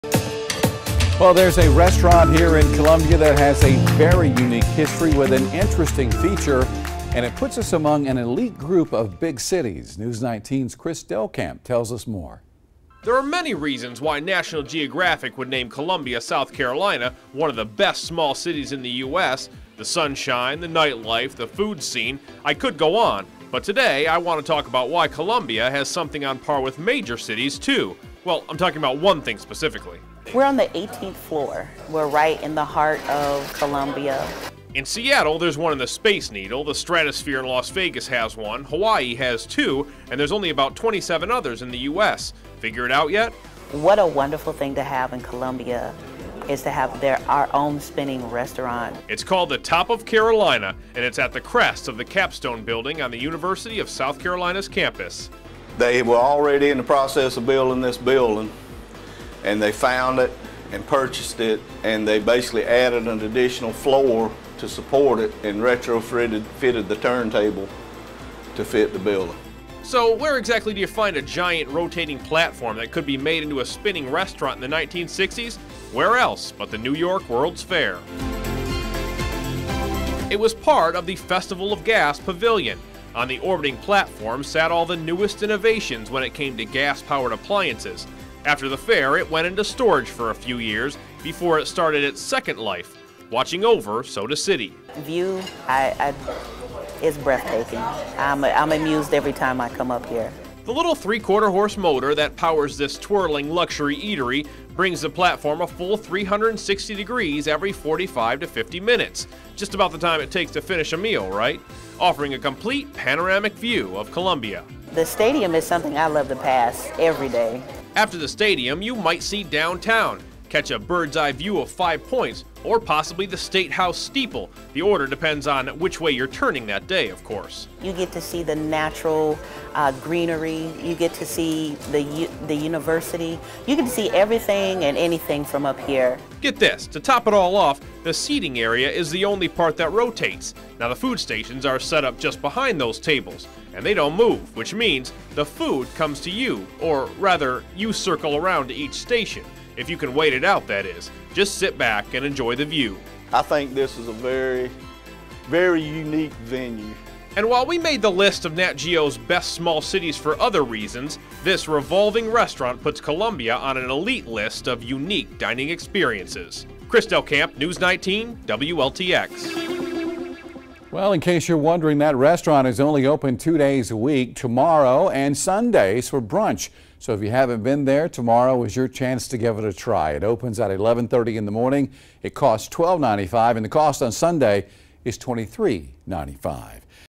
Top of Carolina
There's a restaurant here in Columbia that has a unique history with an interesting feature